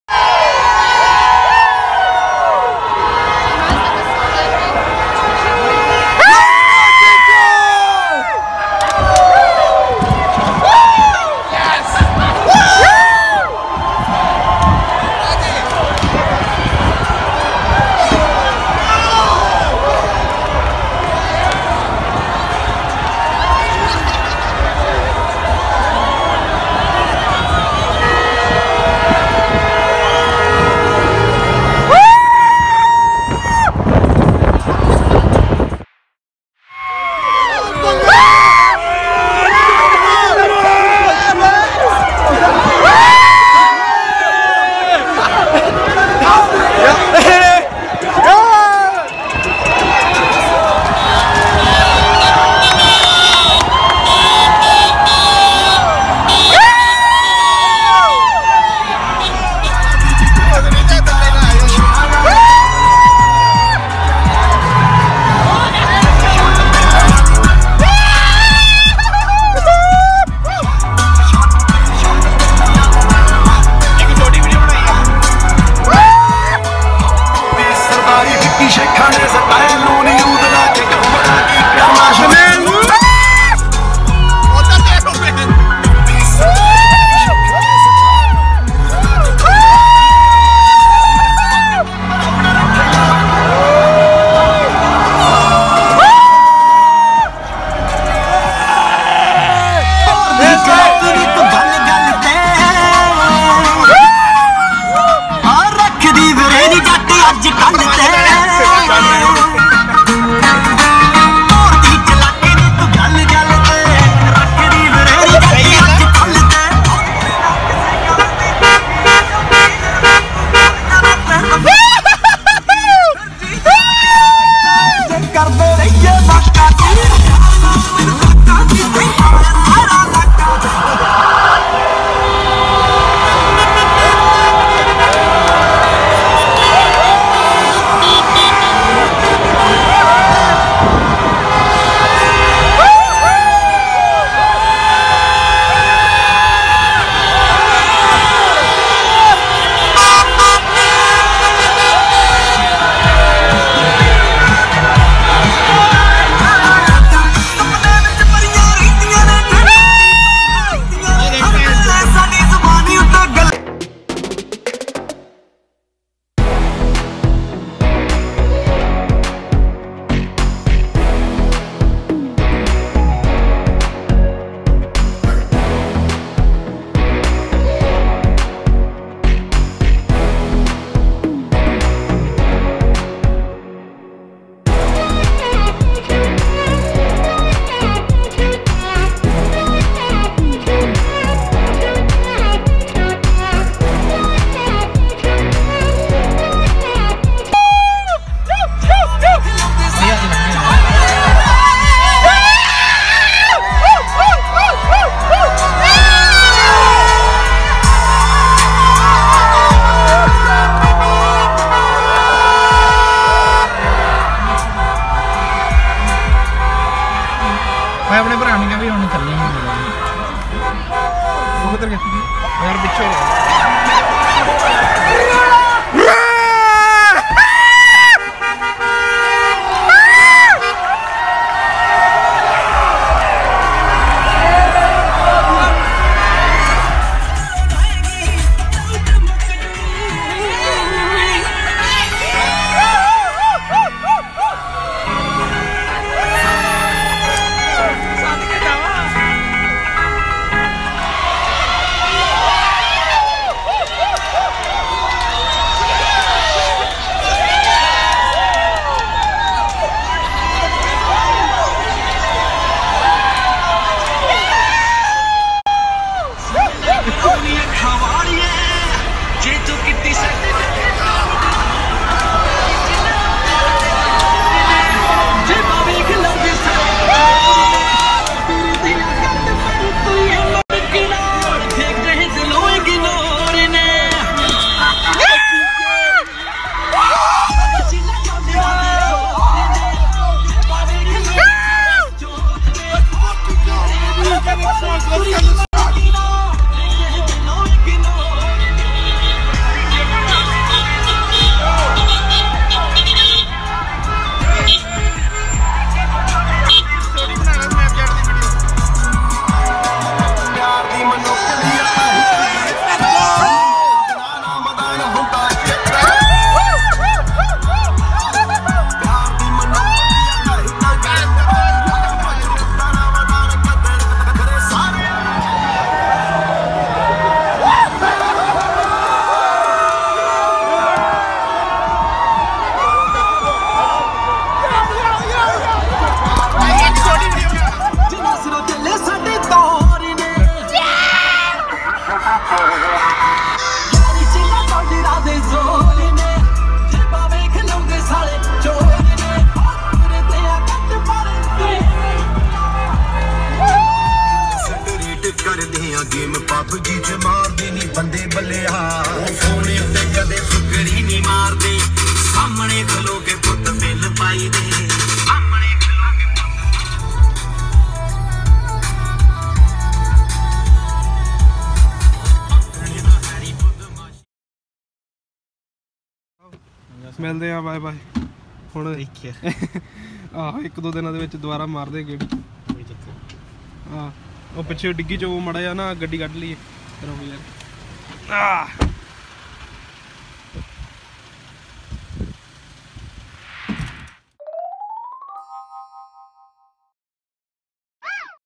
Toronto Raptors Won Basketball Match || Toronto Downtown Crowd || Road Show || Raptors Superfans
Vlog :- 21

Raptors are the 2019 NBA Championship.

The Toronto Raptors defeated the Golden State Warriors 114-110 in Game 6 to win the 2019.